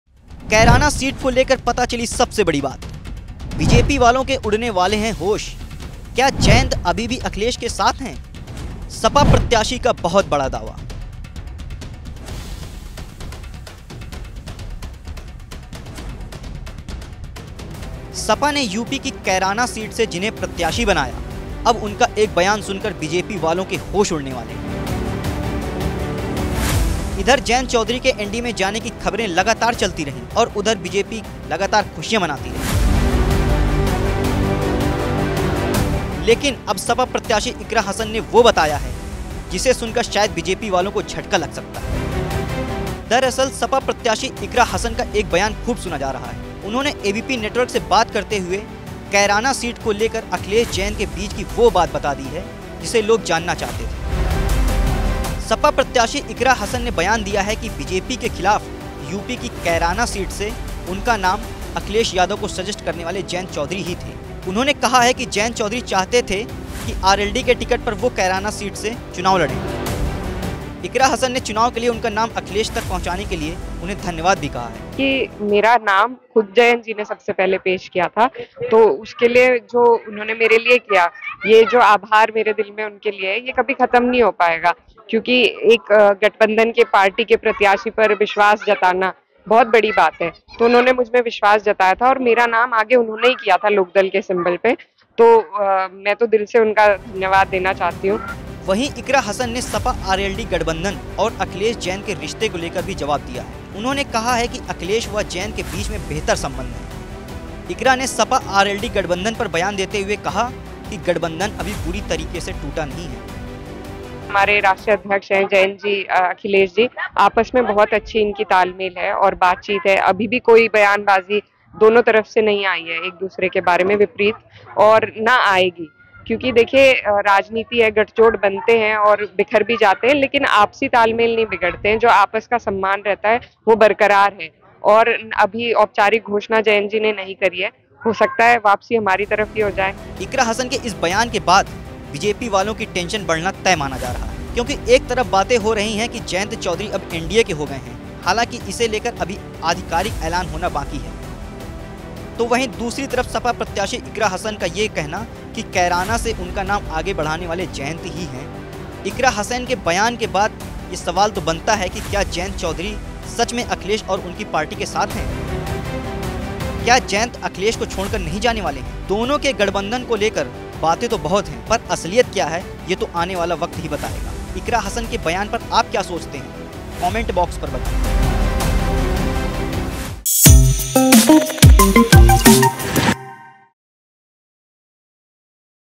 SP-RLD गठबंधन पर Iqra Hasan का बड़ा बयान...अब बढ़ने वाली है बीजेपी की टेंशन ! | Uttar Pradesh
सपा ने यूपी की कैराना सीट से इकरा हसन को प्रत्याशी बनाया है. जिनका एक बयान सुनने से बीजेपी में खलबली हो सकती है.